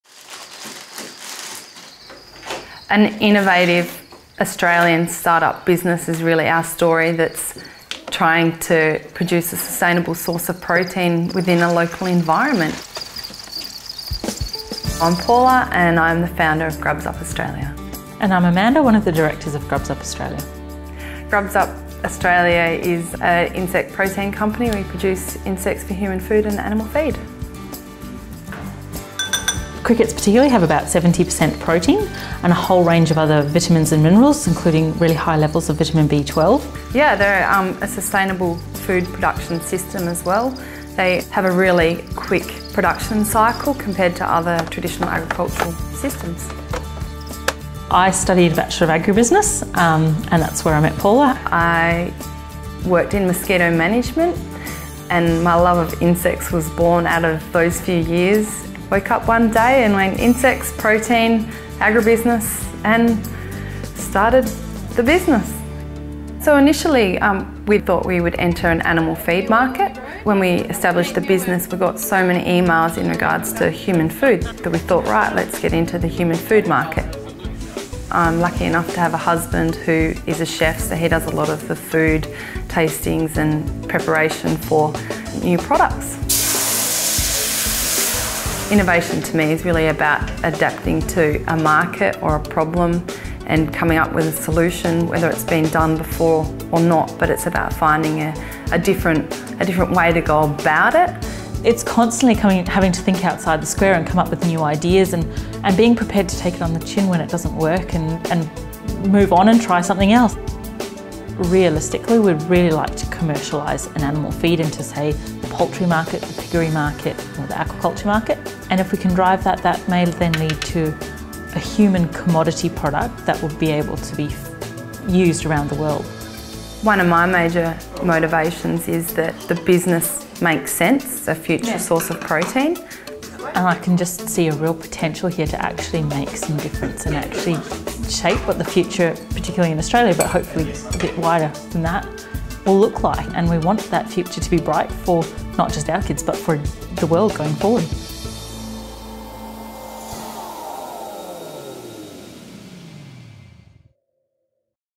Insects, all part of a balanced diet | Curtin Alumni Innovators
Would you eat insects?
Curtin alumni, Paula Pownall and Amanda Abou Rjeilly, the founders of GrubsUp, believe you should. In fact, they believe everyone should.
Check out the story of this innovative startup with the mission of producing a sustainable and healthy source of protein.

to find out more about the inspiring people that make up Curtin University's alumni community.